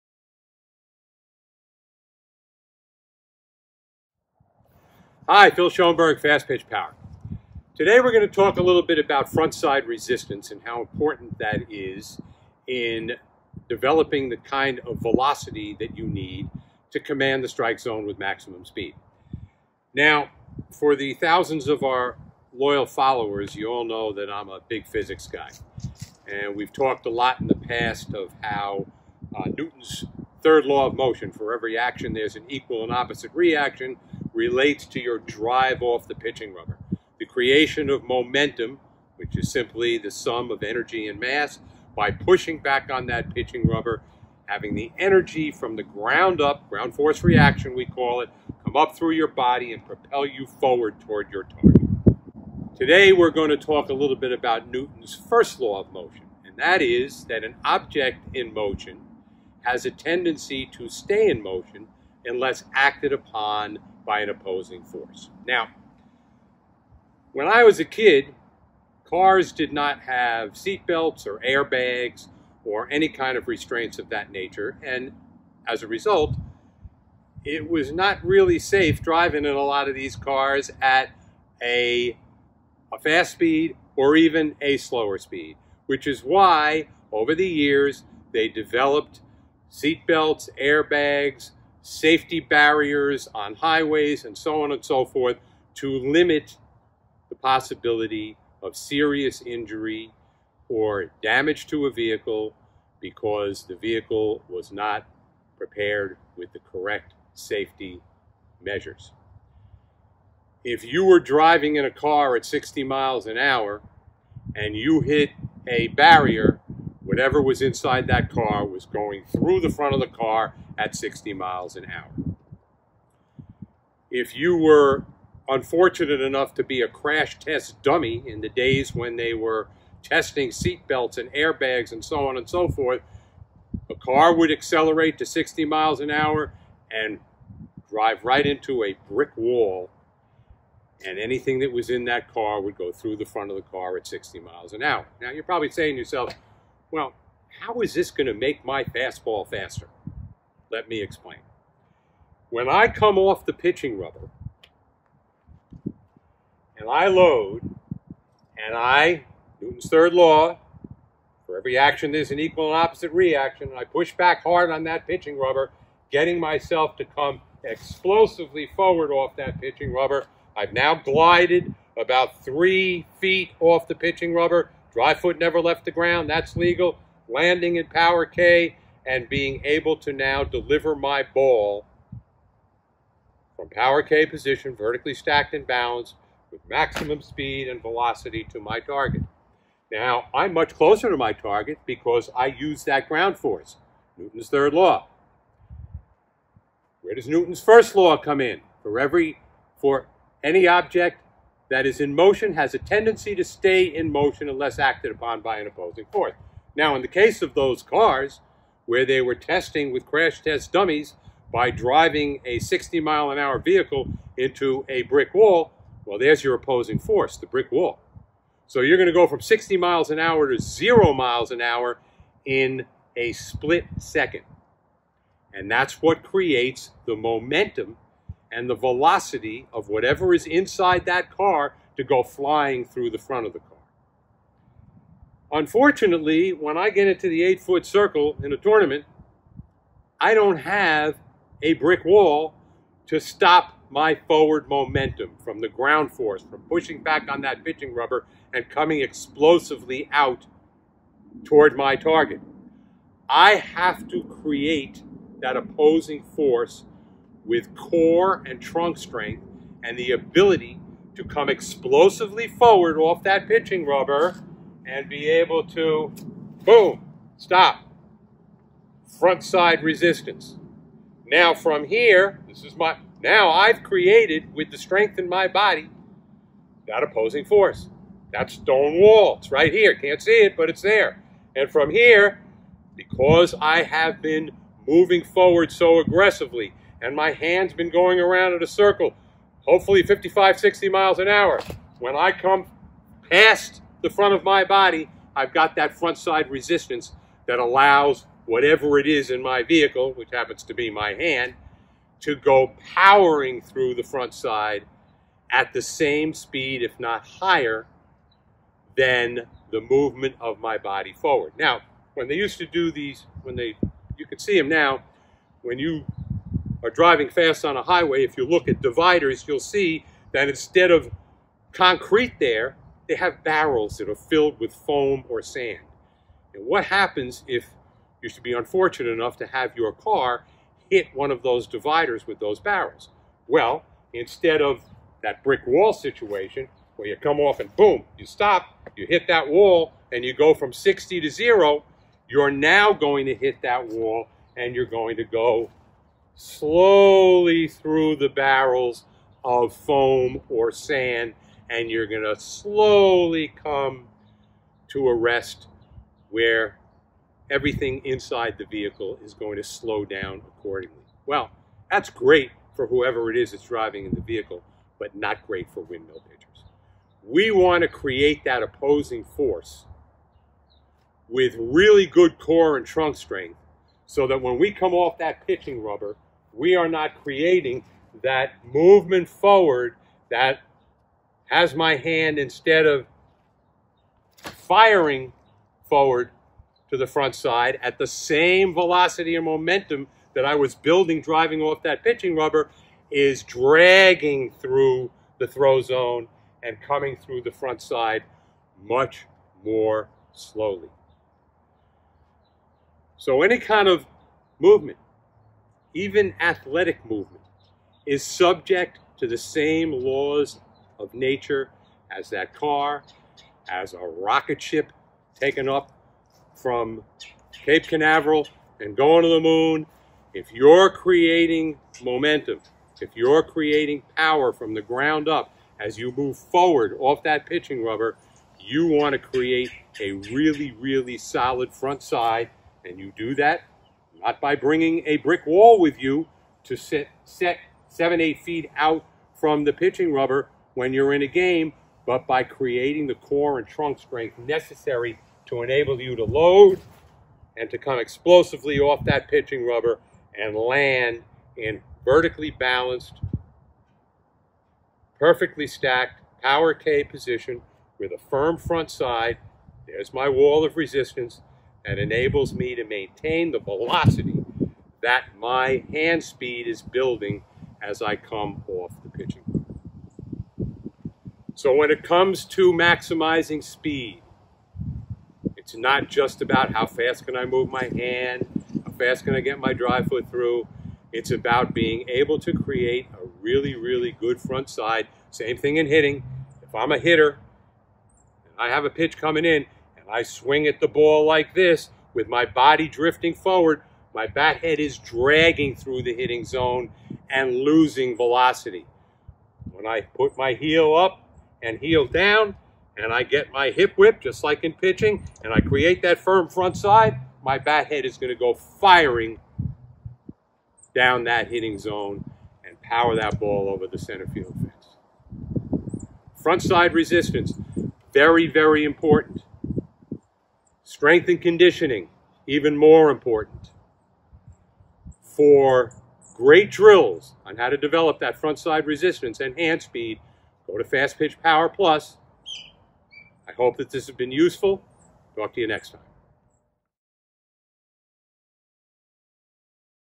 Softball Pitching: Front Side Resistance (And Newton's first law)
Hi everyone! Today Coach Phil is going over the concept of front side resistance and why it is so important when it comes to SPEED in your pitch

Front side resistance is one of the most important parts of the mechanics in pitching, and it takes a lot of core, hip and leg strength to get really good front side resistance. Check out today's video to hear the awesome explanation from coach Phil

Work with us on fastpitchpower.com today! Fastpitch Power+ and Comprehensive Online Clinics available now!

Fastpitch Power+ app available now